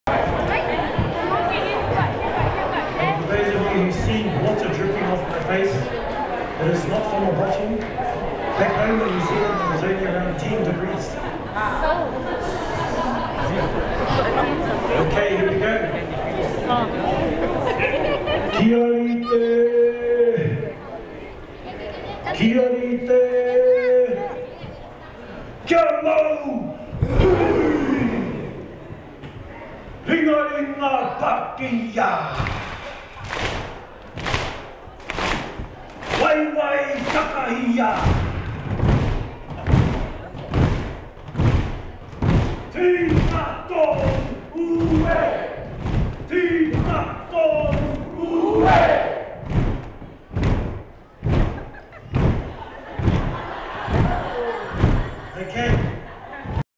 Horomona Horo Haka Workshop at Rainforest World Music Festival 2014
Maori Songs, Dance and Movement Workshop on the Haka and other traditional movement and symbolism at Dewan Lagenda - Rainforest World Fest Day 3.

Had a lot of fun at this workshop, just a minute of video and then having fun the rest of the workshop. :D Haka is so cool!